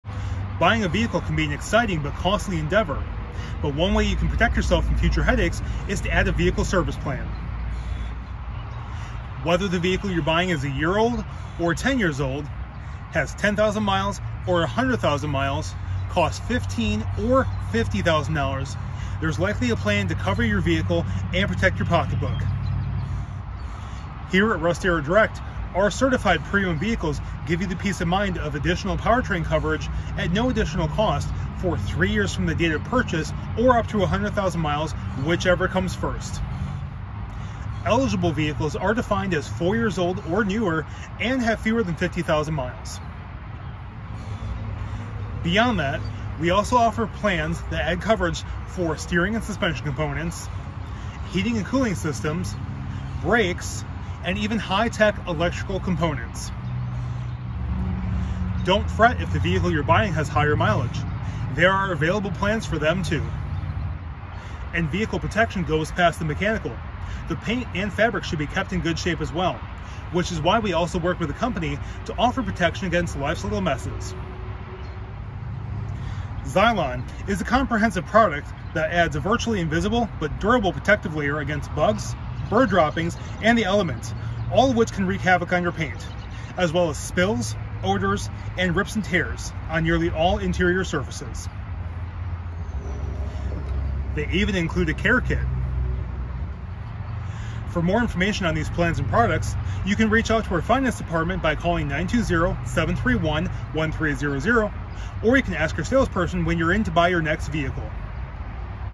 Russ Darrow Direct Vehicle Service Contract
Brennan reviews Russ Darrow Direct's Vehicle Service Contract's. We want you to be able to protect your investment when you purchase your vehicle.